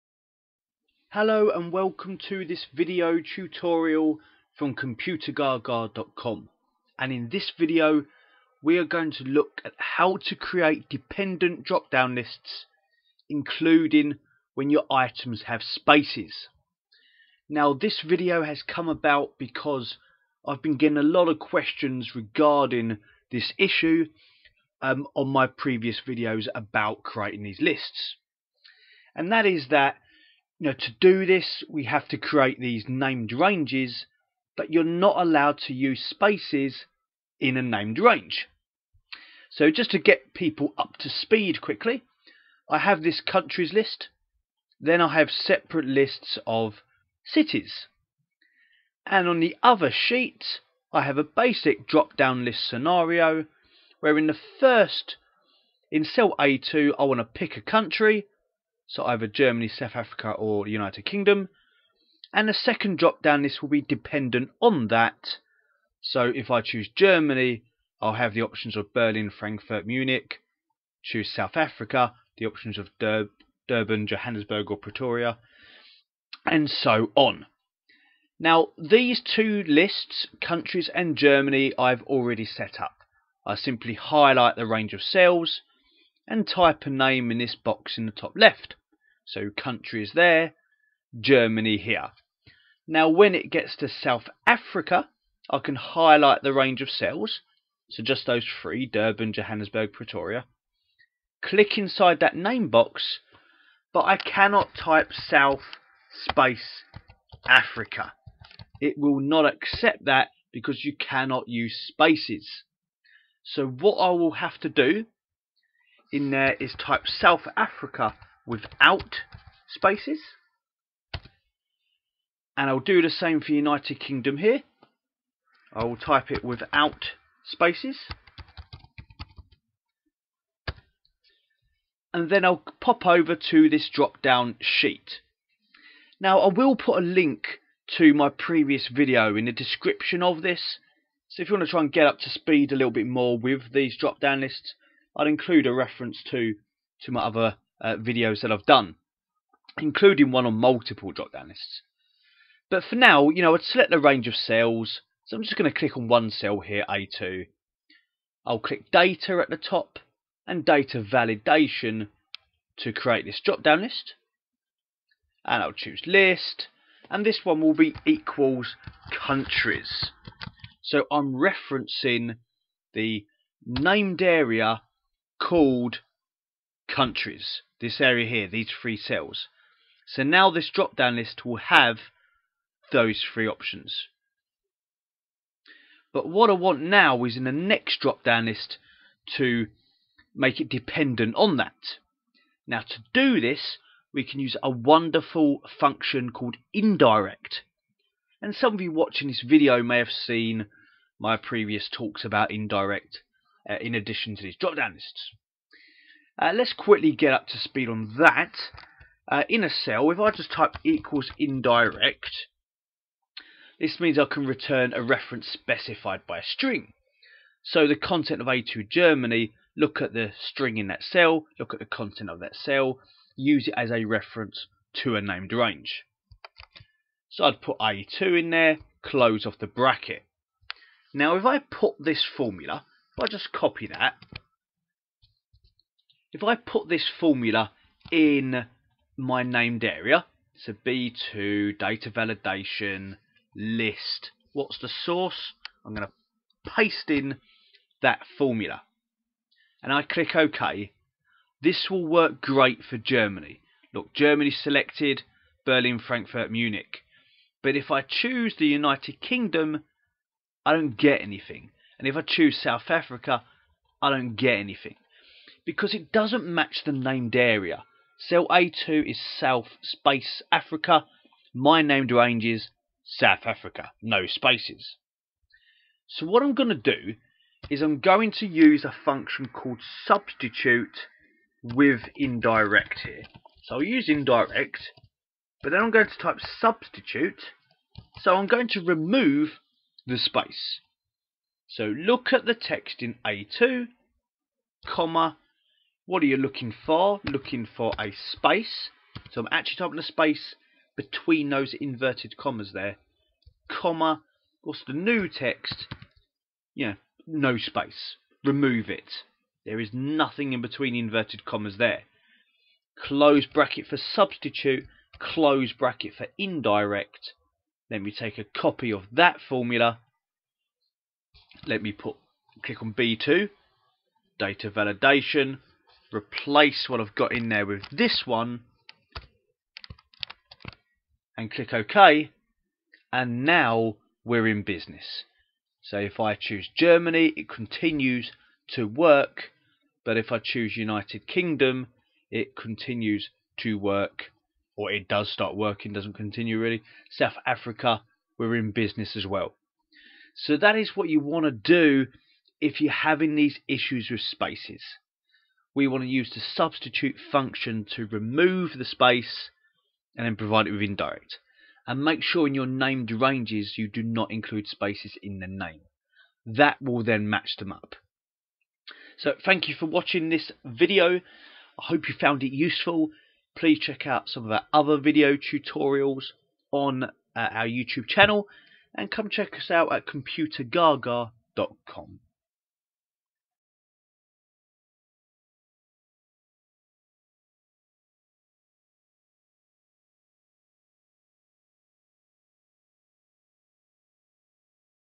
Problem Solved: Dependent Drop Down Lists in Excel
In this video we answer a common problem people have when creating dependent drop down lists in Excel. And that problem is when your list items contain spaces.

This question has been asked of me many times since creating my dependent drop down lists video

To create these lists we need to set up named ranges. However named ranges do not allow the use of spaces in a name.

So how can we still use this technique? If the named range does not have a space, but the item on the worksheets does. This video shows you how.